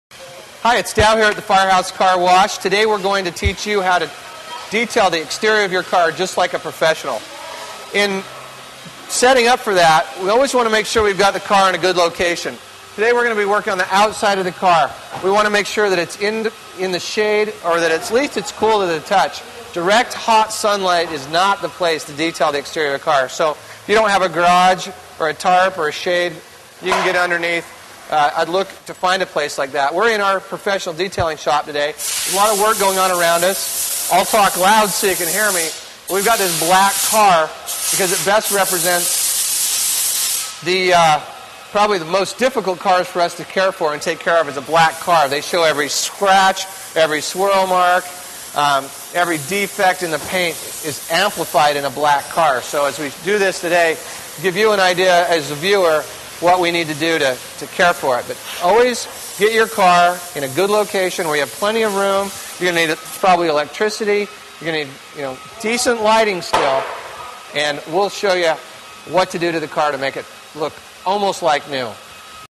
Car Detailing Locations
Car Detailing Locations. Part of the series: Exterior Car Detail Tips. When detailing a car exterior, place the vehicle in a shaded location and away from sunlight. Learn more about car detailing locations from the owner of a car detail shop in this free video on exterior car details.